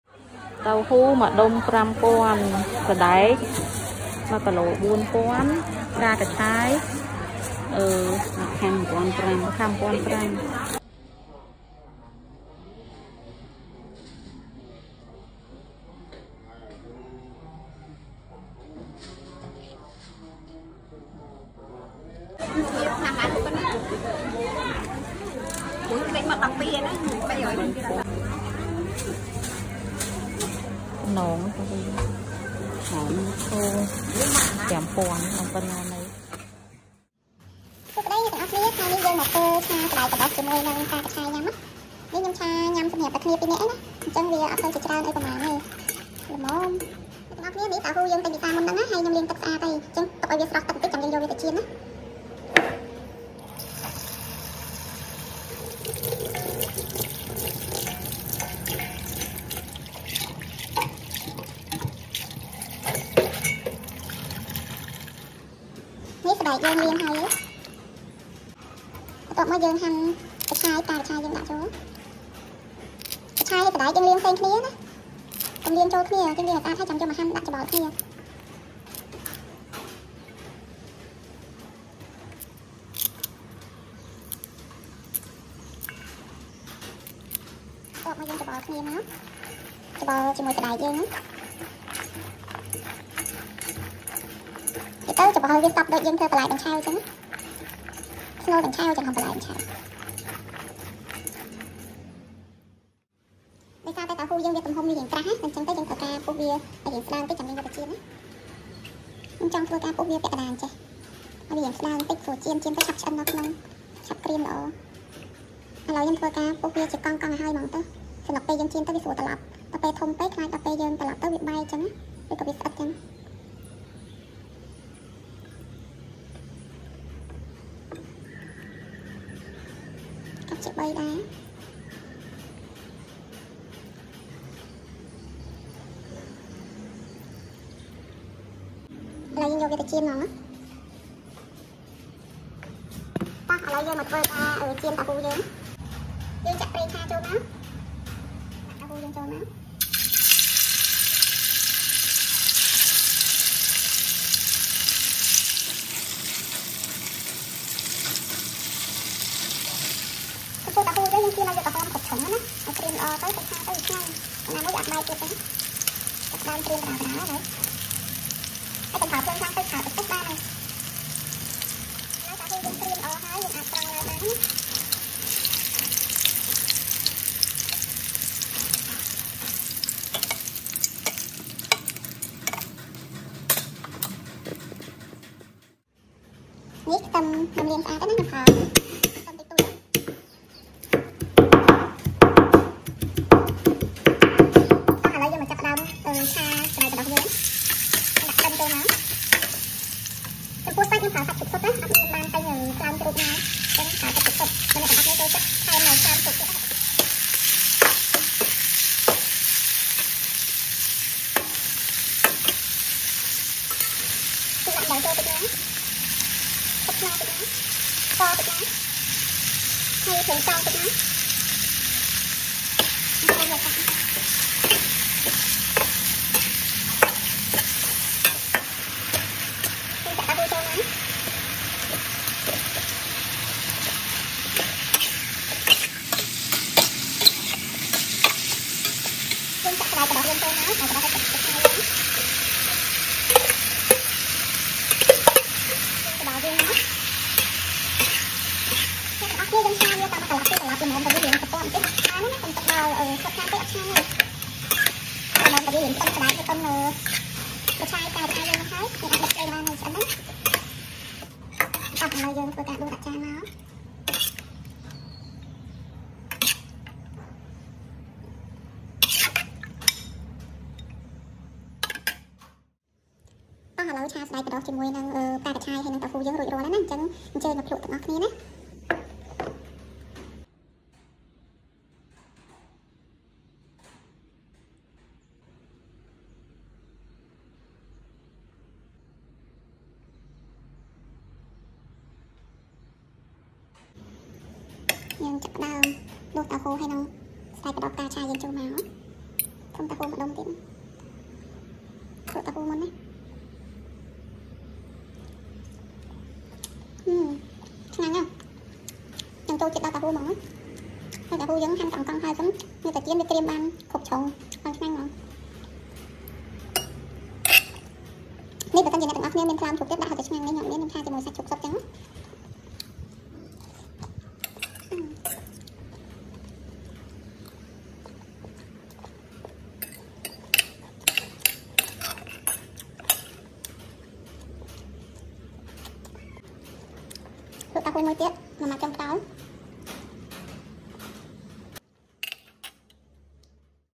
How to Fry Garlic Chives with Tufo and Pork - Garlic Chives Recipe - Yummy Cooking Vlogs
Hi Everyone,
Please enjoy with this video. Thanks!

Love From Cambodia
Sophea,

My Dad Pick Coconut for Us - Natural Coconut - How to Cut Coconut Properly - Yummy Cooking Vlogs